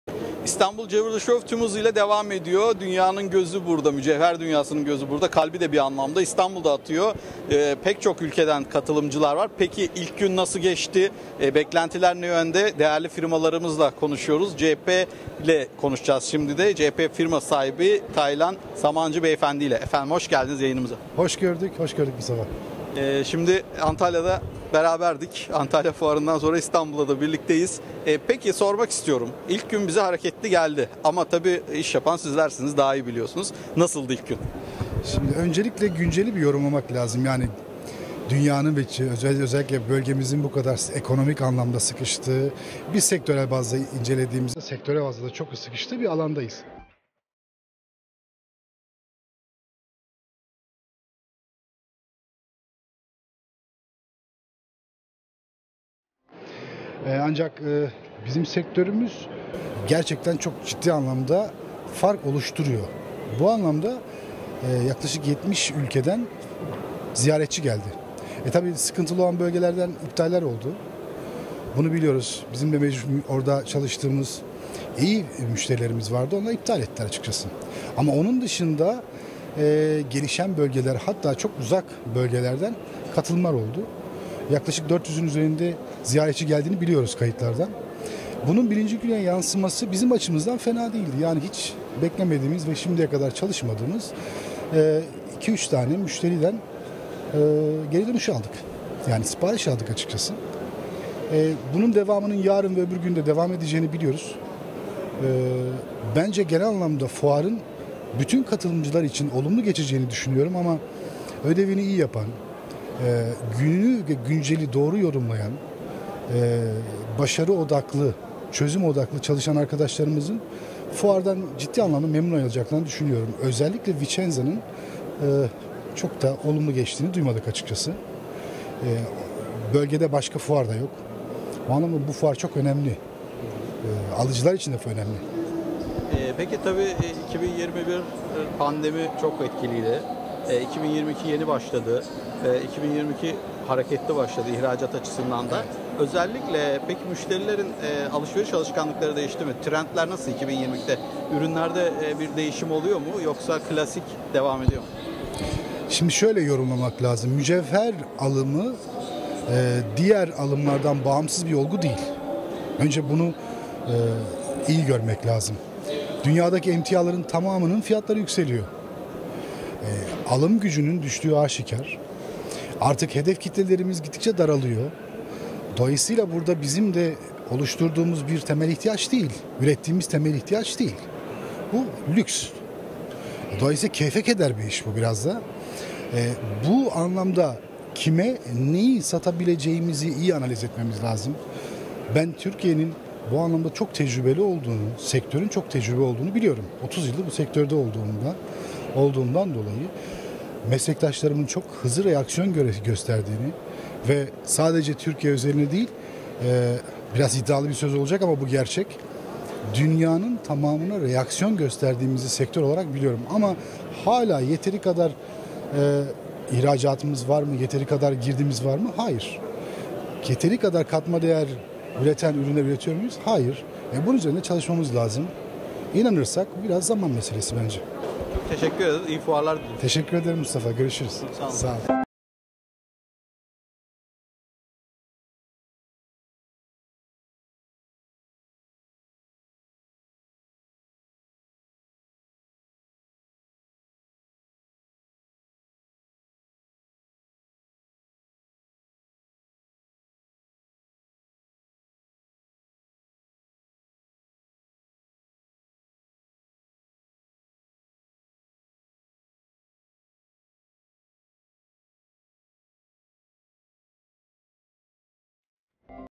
JP Firma Sahibi Taylan Samancı İstanbul Jewelry Show'u Değerlendirdi
51'inci İstanbul Mücevher Fuarı-Istanbul Jewelry Show renkli görüntülere sahne oldu. JP Firma Sahibi Taylan Samancı, fuarı değerlendirdi

 KATILIMCILAR MEMNUN AYRILDI

Fuarda, altın, mücevher, pırlanta, renkli taş, değerli ve yarı-değerli taş, inci, gümüş, gümüş takı ve gümüş ev eşyası, altın montür, saat, kalıp, vitrin dekorasyon, makine-ekipman ve yan sanayi, kasa, yazılım, lojistik ile aydınlatma ürün gruplarının bulunduğu 1.250'nin üzerinde yerli ve yabancı mücevher firma ve markası yer alırken, katılımcı firmalar özel ürün ve koleksiyonlarını tüm dünyadan yaklaşık 30 bin sektör profesyonelinin beğenisine sunuldu.